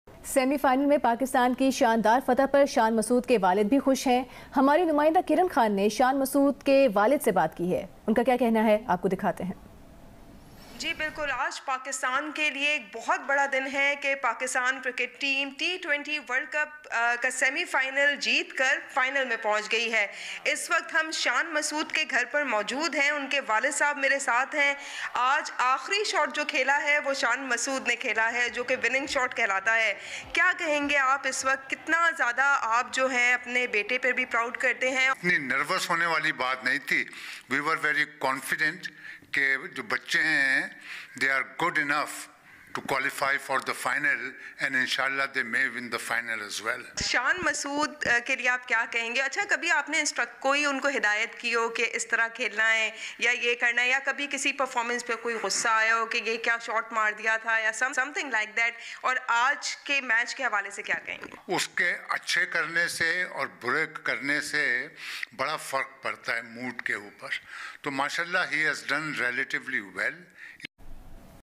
Pakistan beats New Zealand - Exclusive conversation with Shan Masood's father | Geo Super
Pakistan beats New Zealand - Exclusive conversation with ShanMasood's father | Geo Super